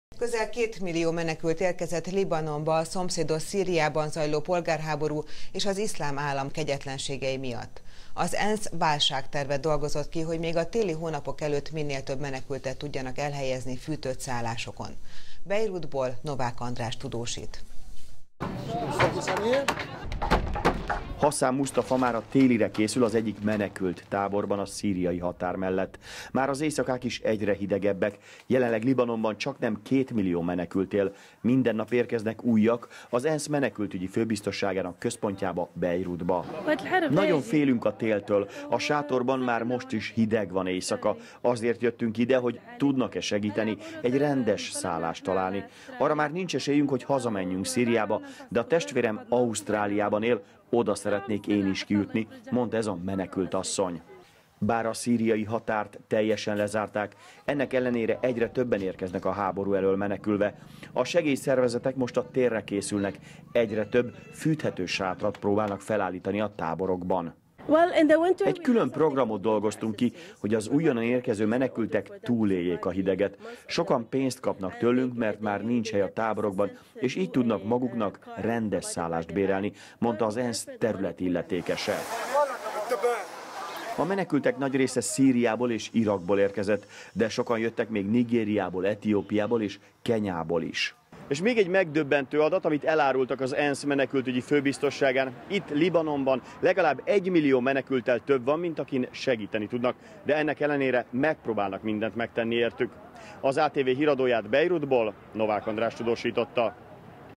A télre készülnek a menekülttáborokban
Csaknem két millió menekült érkezett Libanonba a szomszédos Szíriában zajló polgárháború és az Iszlám Állam kegyetlenségei miatt. Az ENSZ válságtervet dolgozott ki, hogy még a téli hónapok előtt minél több menekültet tudjanak elhelyezni fűtött szállásokon.